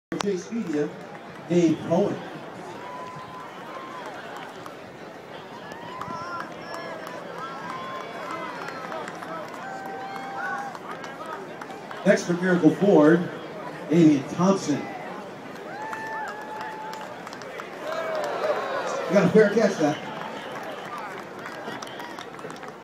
David Cohen hurls first pitch at Renegades game
Chase Media Group controller David Cohen had the honor of throwing the ceremonial first pitch on Chase Customer Appreciation Night July 16, 2010, when the Hudson Valley Renegades, Tampa Bay Rays' Single-A minor league team in Fishkill, took on the Williamsport Crosscutters, part of the Philadelphia Phillies franchise.